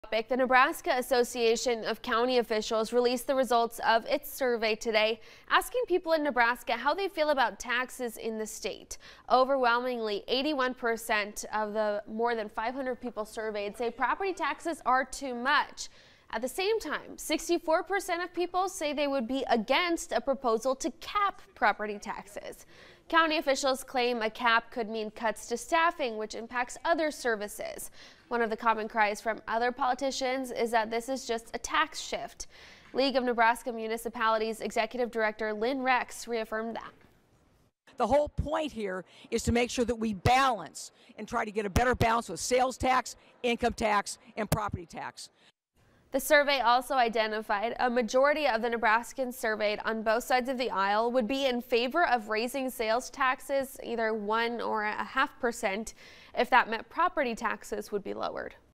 Poll: Nebraskans in dire need of property tax relief support sales tax increase but not property...
Members of the Nebraska Association of County Officials (NACO) met at the State Capitol to share the results of their tax polling on Thursday.